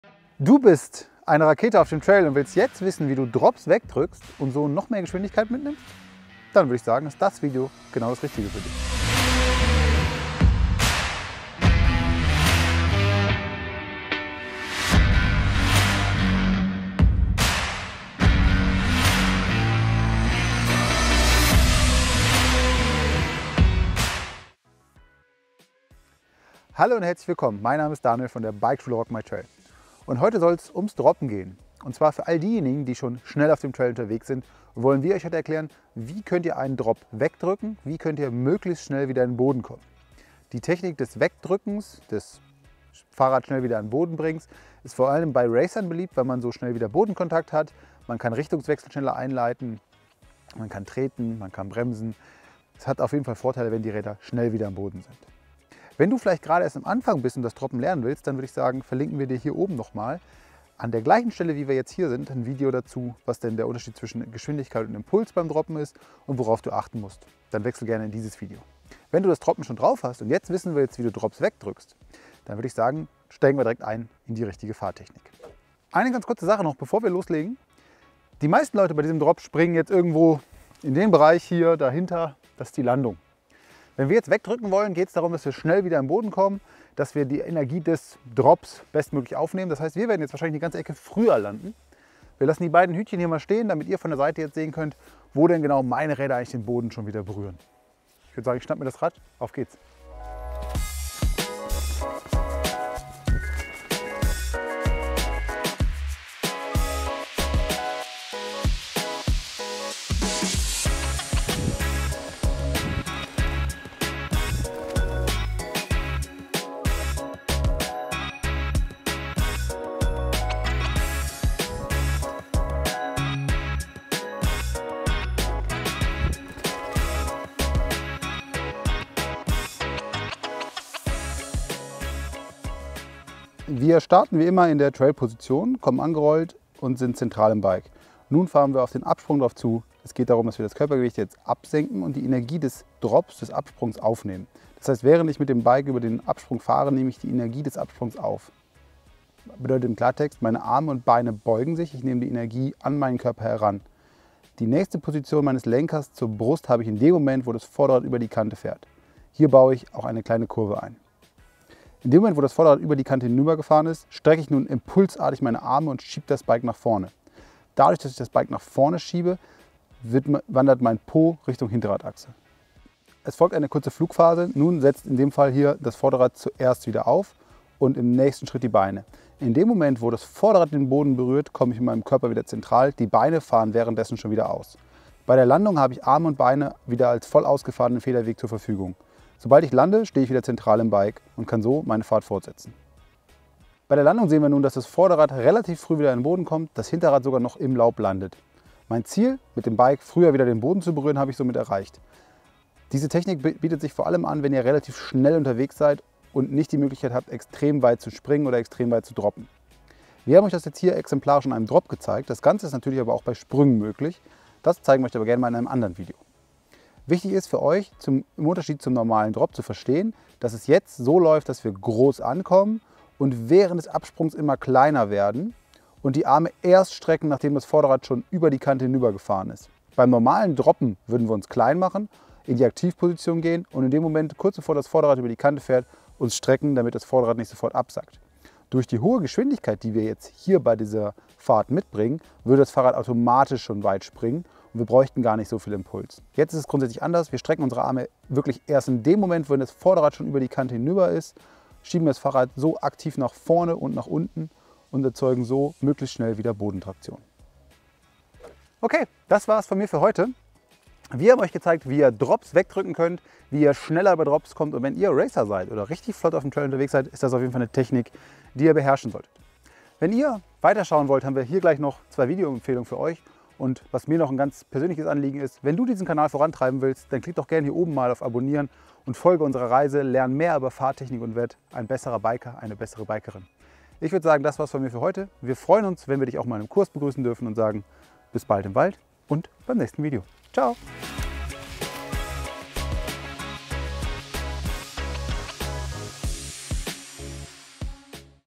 Fahrtechnik für Profis - Wegdrücken & schneller über Drops - Tipps für den Mini-Scrub
Du bewältigst Drops mit verschiedenen Höhen, willst aber nicht mehr davor Bremsen? Dann los gehts! Hier kommt die Technik um Drops wegzudrücken, schnell zu bleiben und sicher mit Speed den Trail runterzukommen.

Du brauchst individuelles Feedback, einen Trainer, geeignetes Übungsgelände und eine Videoanalyse? Dann komm zu einem unserer Fahrtechnik Kurse.